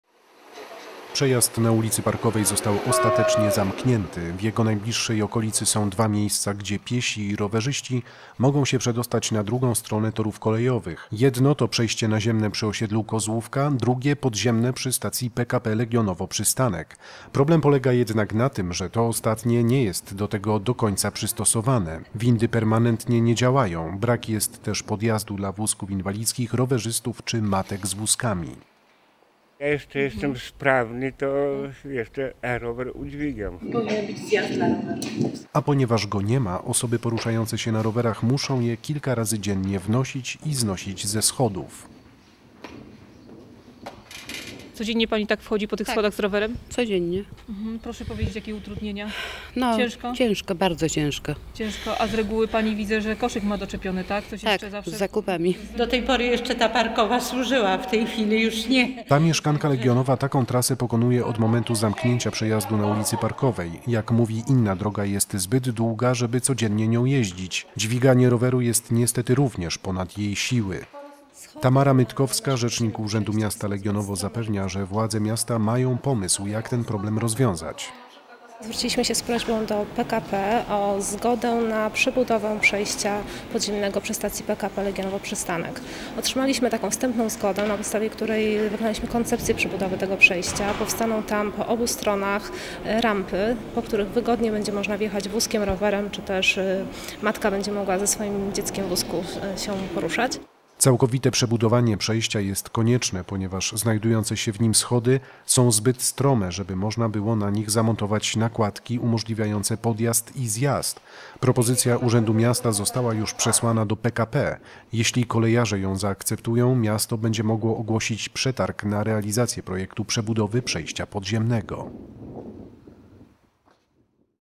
Przebudują przejście ?
Przejazd na ul. Parkowej został ostatecznie zamknięty. W jego najbliższej okolicy są dwa miejsca, gdzie piesi i rowerzyści mogą się przedostać na drugą stronę torów kolejowych. Jedno to przejście naziemne przy osiedlu Kozłówka, drugie – podziemne przy stacji PKP Legionowo Przystnaek. Problem polega jednak na tym, że to ostatnie nie jest do tego do końca przystosowane. Windy permanentnie nie działają. Brak jest też podjazdu dla wózków inwalidzkich, rowerzystów czy matek z wózkami.